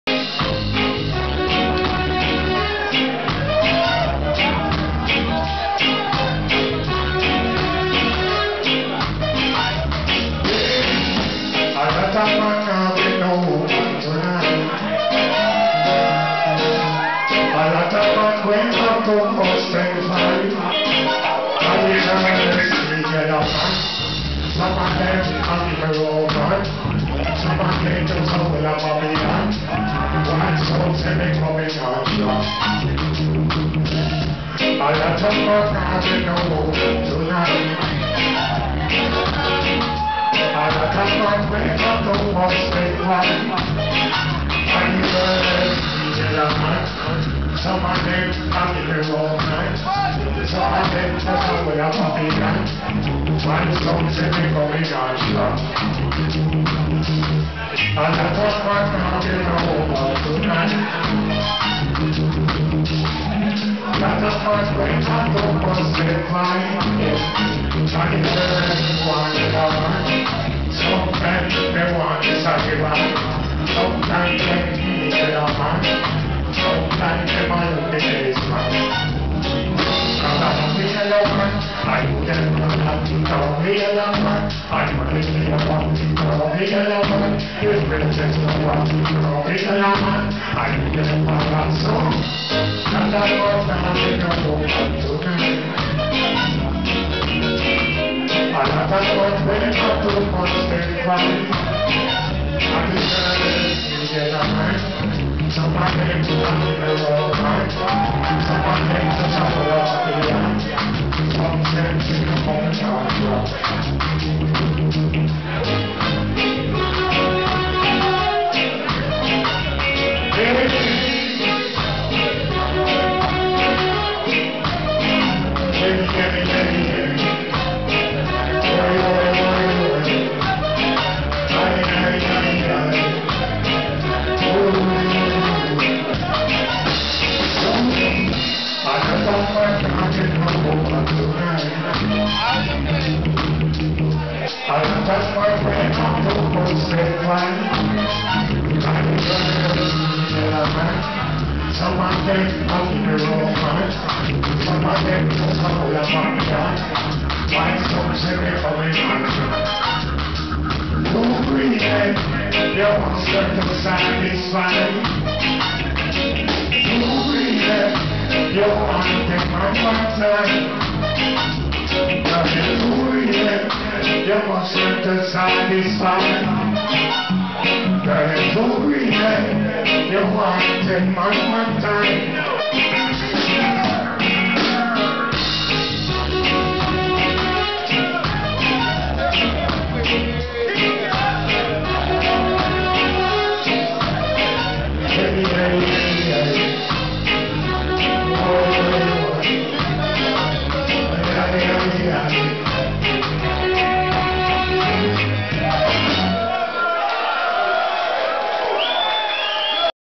YELLOWMAN
Performed like the early days of reggae/dancehall. YELLOWMAN is a legend!